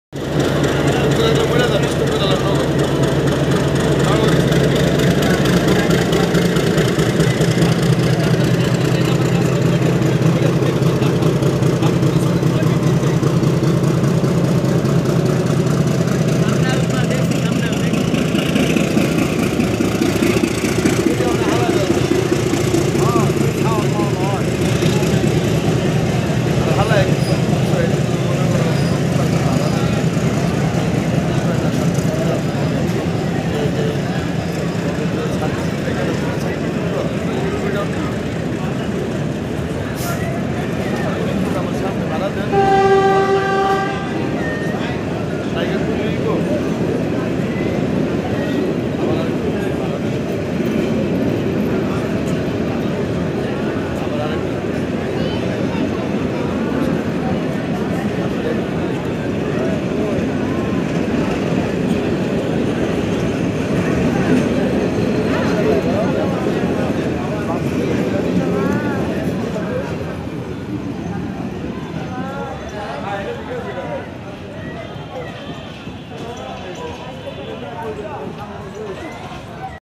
RIDING THE RAILS || Experience the Jamalpur Commuter Train in Bangladesh
Discover the beauty of Bangladesh's railway system with a ride on the Jamalpur commuter train. This authentic adventure takes you through stunning countryside, bustling cities, and historic sites, providing a unique perspective on the country's rich culture and history. Follow us on this journey as we explore the sights, sounds, and people of the Jamalpur commuter train.